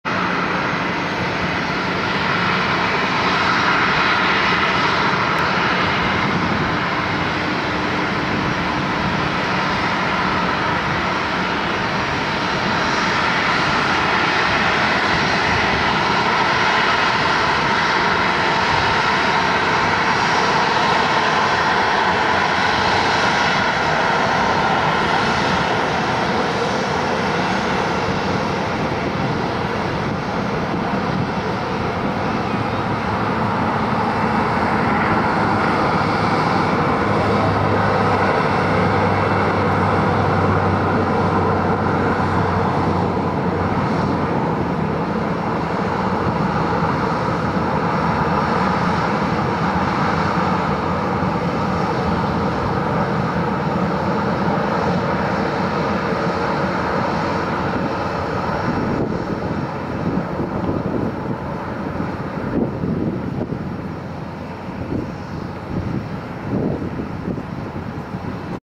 夜の中部国際空港 中国東方航空(雲南公司) B-5527
中国東方航空・雲南公司と描かれた機体初めて見た。